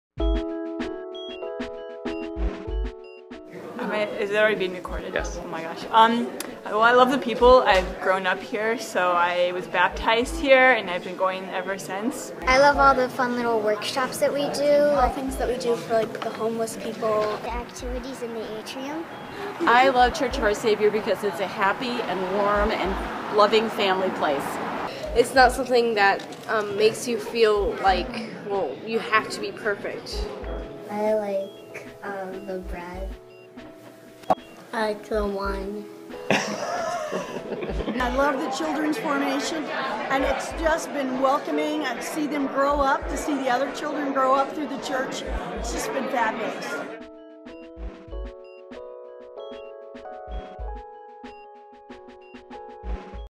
Kids at Church of Our Saviour: Lincoln Park's Episcopal Church
Hear from the children and families at Church of Our Saviour, and see why we love it so much!